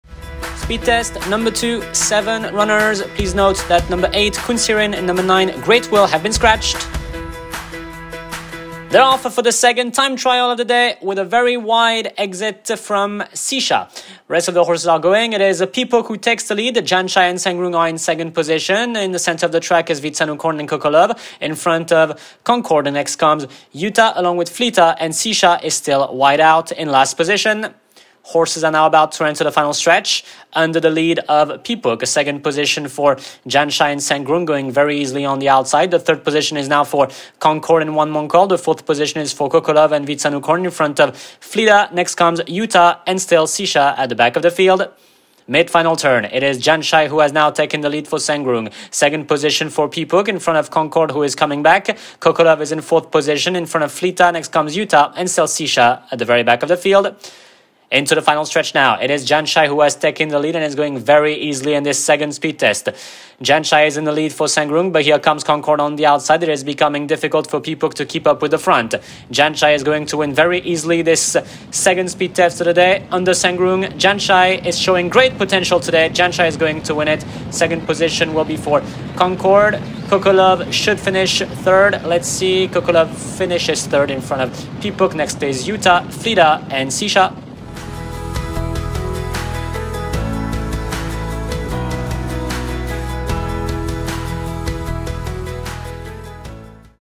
Time Trial 2 (Speed Test) - 06/09/2023
Time Trial 2 - 06/09/2023
1. Concorde - 1:13.47
2. Sicha 1:18.80
3. Fleeta 1:17.40
4. Jan Chai 1:12.75
5. Utah 1:16.75
6. Pee Pook 1:16.63
7. Coco Love 1:15.69
8. Khun Sirin (SCR)
9. Great Will (SCR)